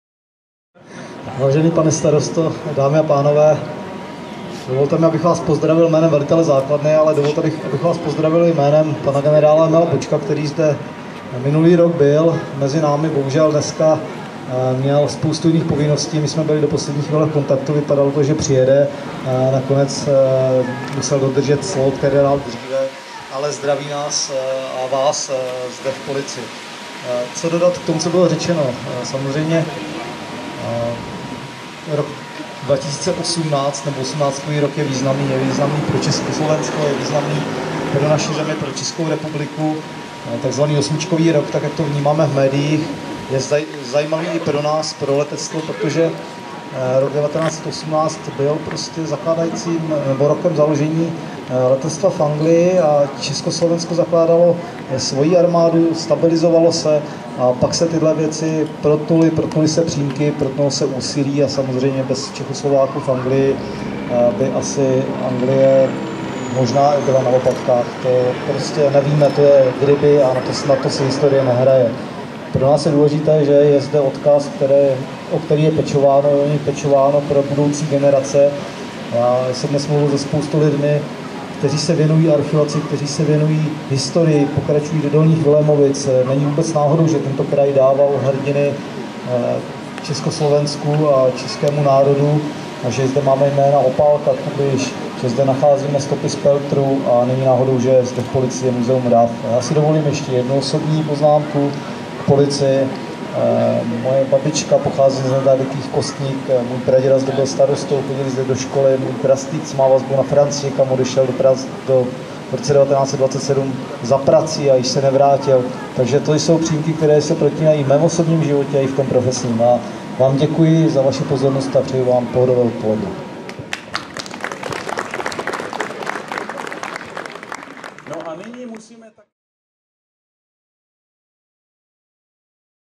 RAF v Polici - 23.6.2018
Projev zástupce velitele plk.gšt.Ing. Miroslava Svobody, při otevření muzea RAF v Polici, 23.června 2018.